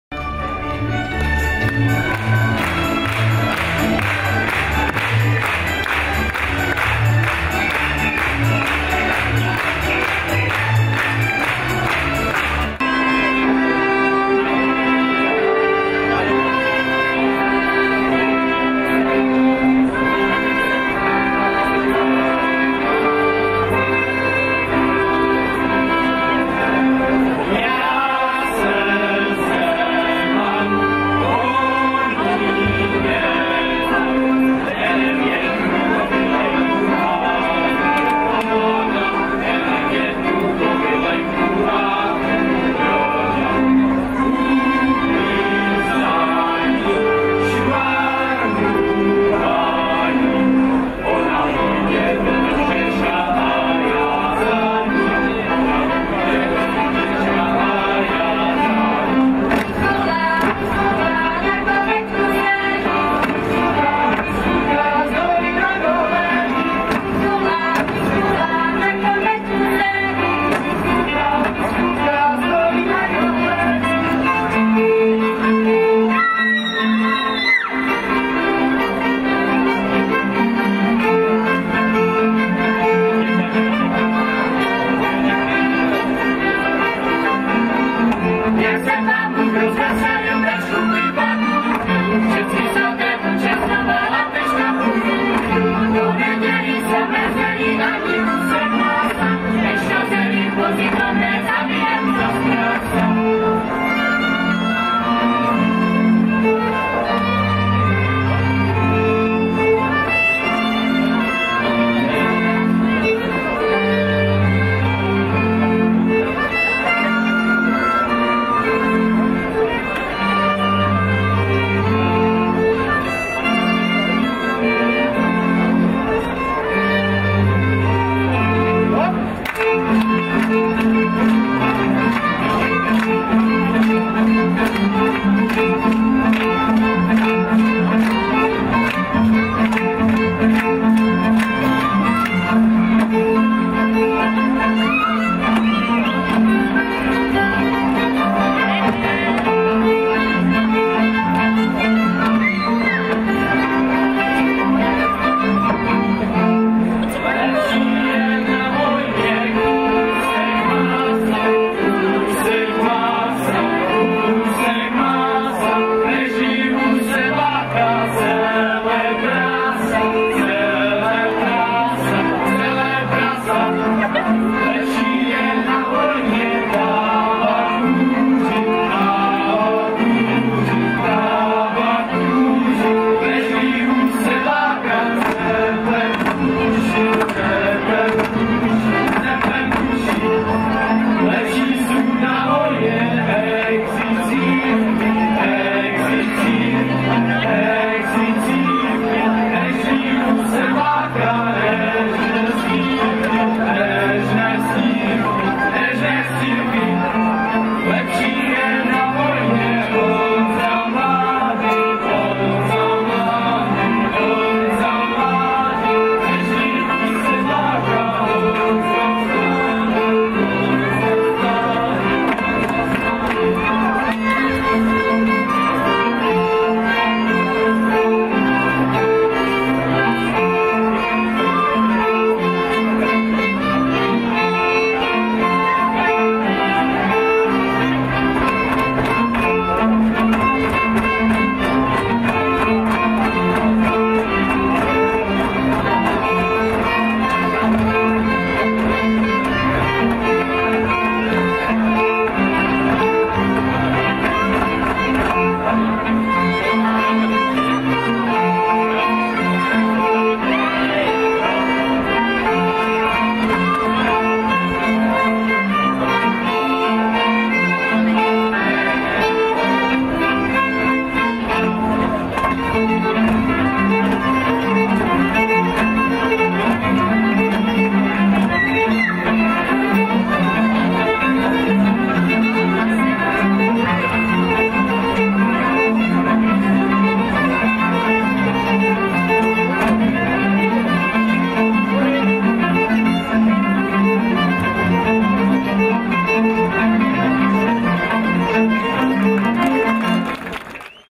Dětský folklorní soubor Ostravička, Frýdek-Místek (2)
17. Mezinárodní folklórní festival CIOFF Frýdek-Místek 2011.

17th IFF CIOFF Frydek-Mistek 2011. The Final concert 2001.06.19.

17. Międzynarodowy Festiwal Folkloru CIOFF Frydek-Mistek 2011.